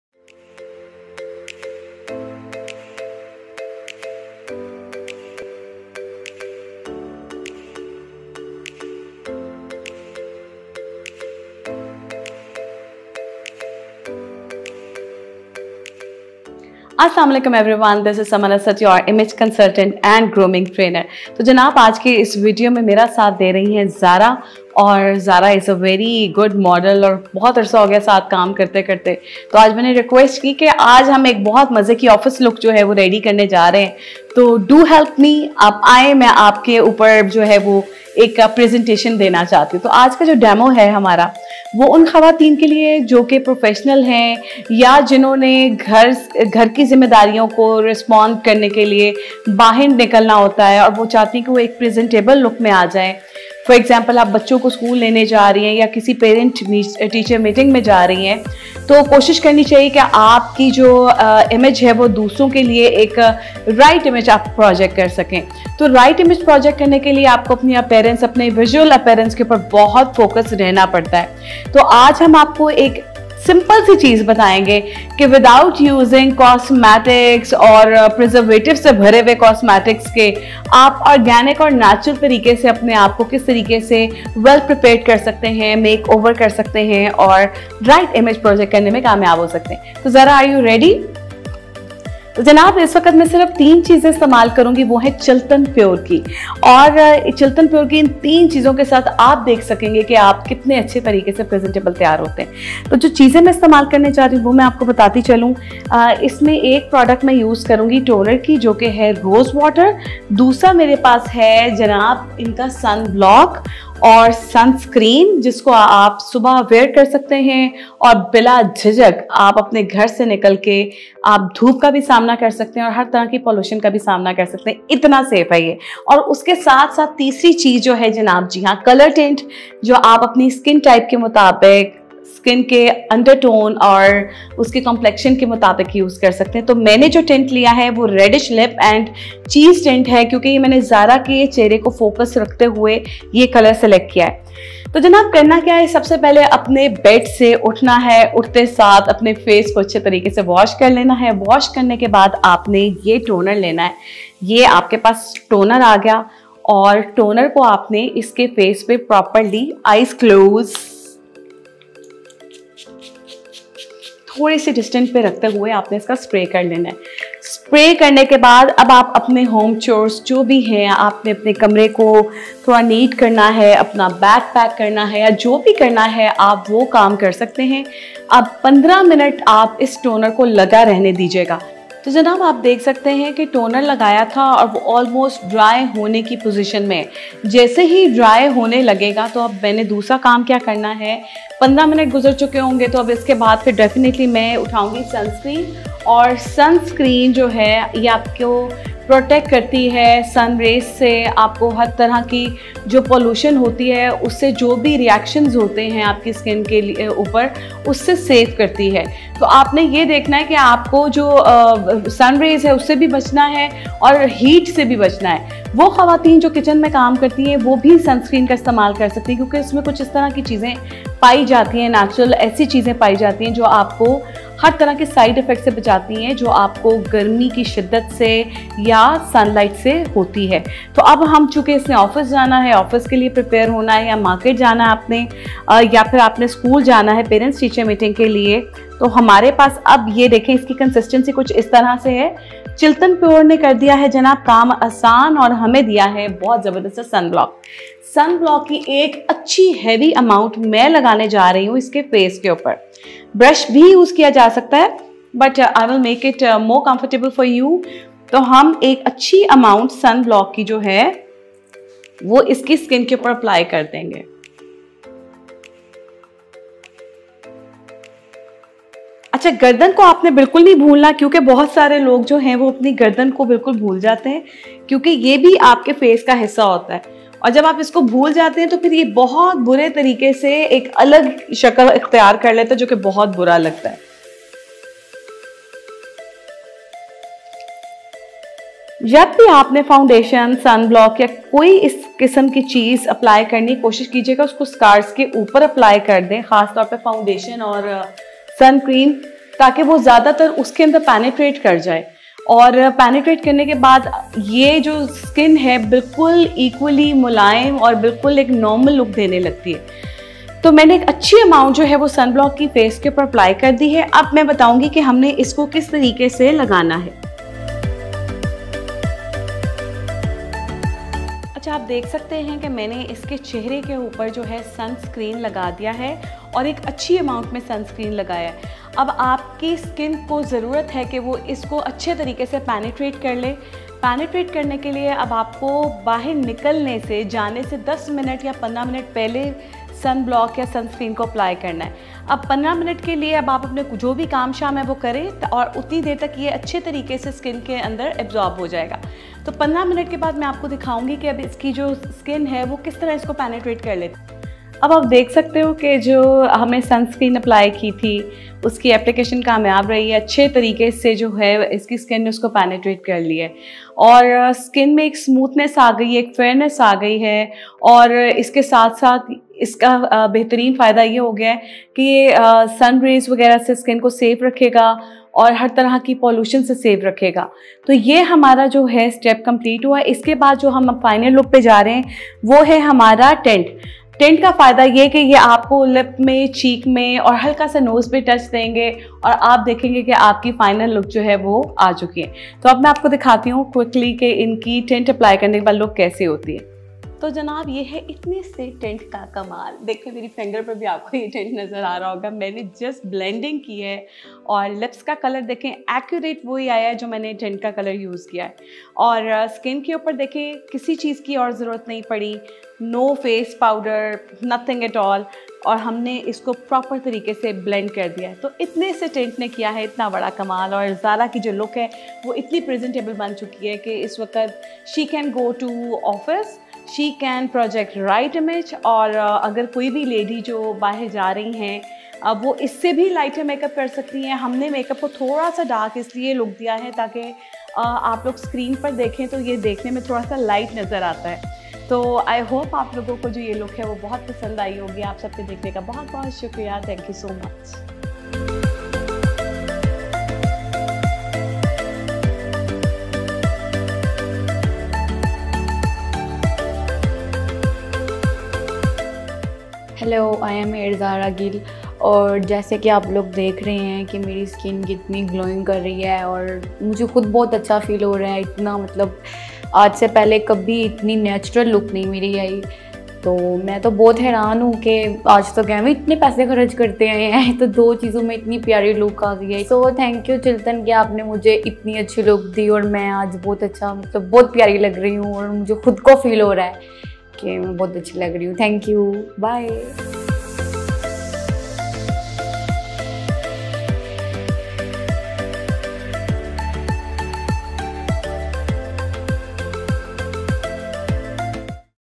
How to get ready with just 3 products
Use of toner, sun screen and tint will make you presentable with no makeup look.